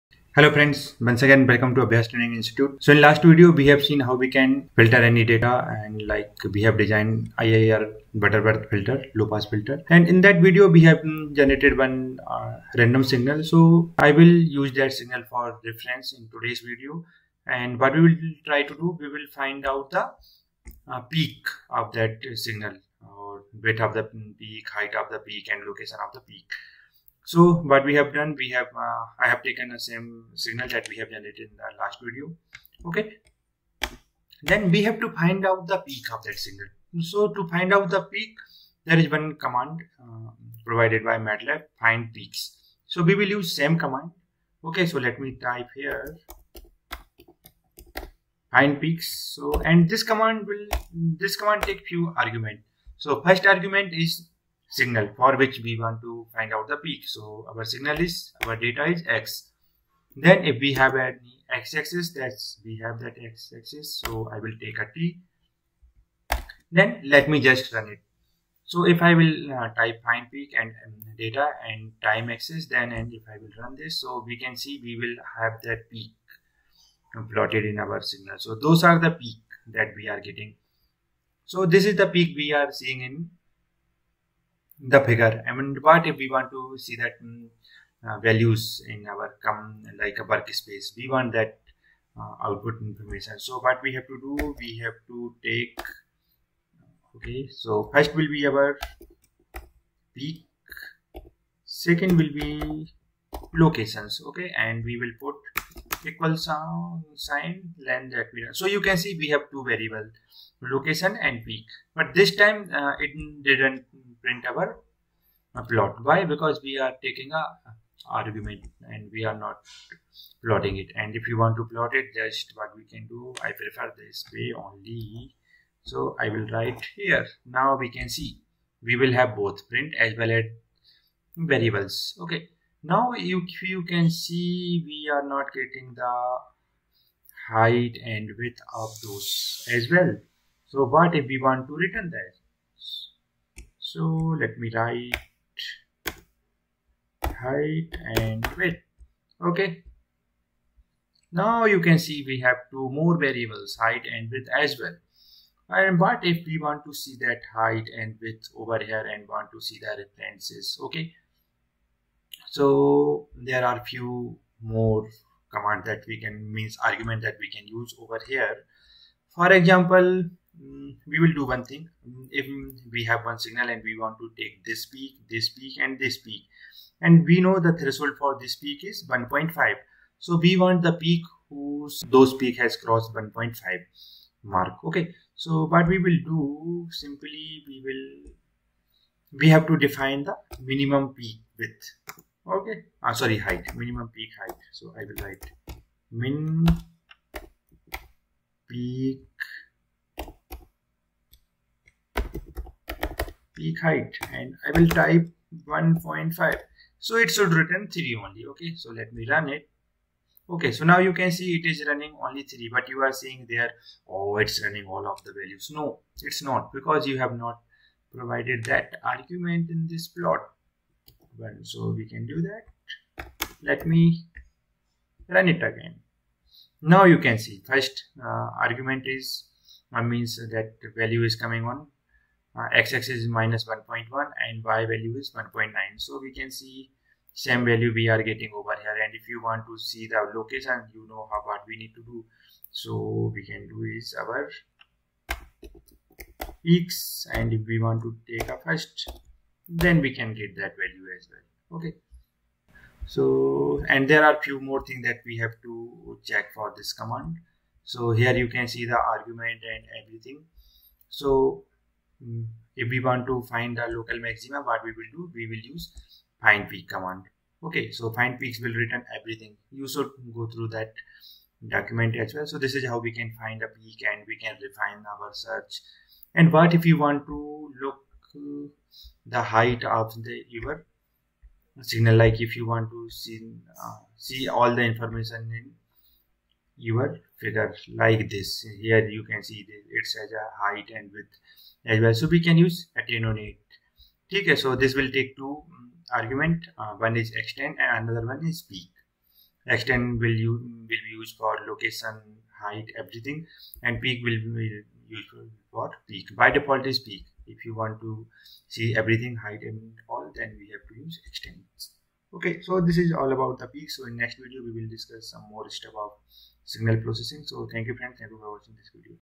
Find Peak of Signal in MATLAB | Peak Widths, Height, Location of Signal | findpeaks | P- 20
Outline:-
 Find Peaks and their locations in MATLAB, Peak Height, Peak Width in MATLAB.
Plot peak Annotate.
Signal Processing

Let's practice and get trained in MATLAB, Simulink, LabVIEW, FPGAs, VHDL and Verilog languages, Arduino, NodeMCU, Robotics, and Python.